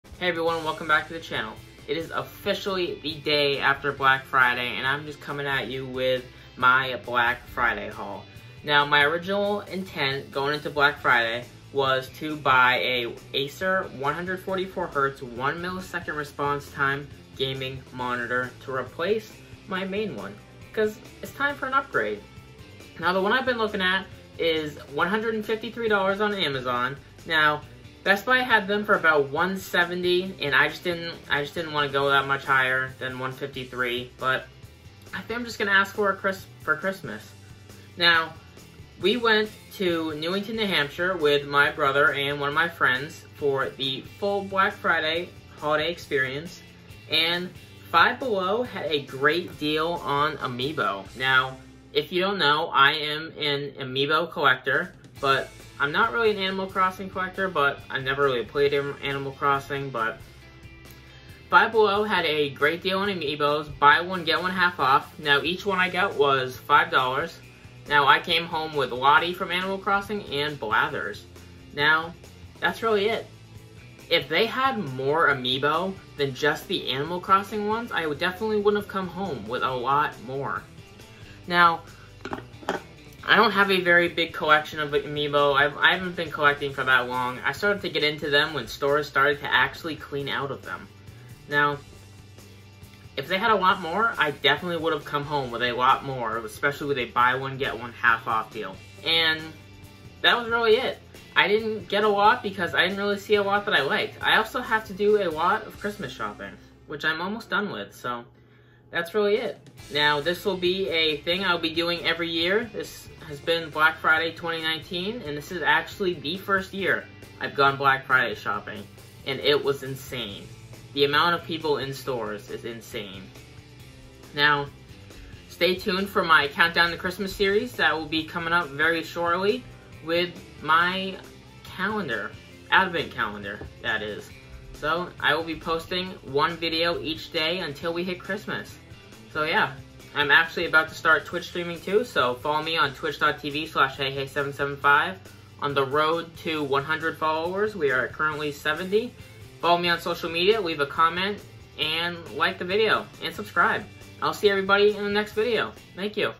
Black Friday Haul 2019
Taking a look at what I bought for Black Friday 2019!

Catch my twitch streams!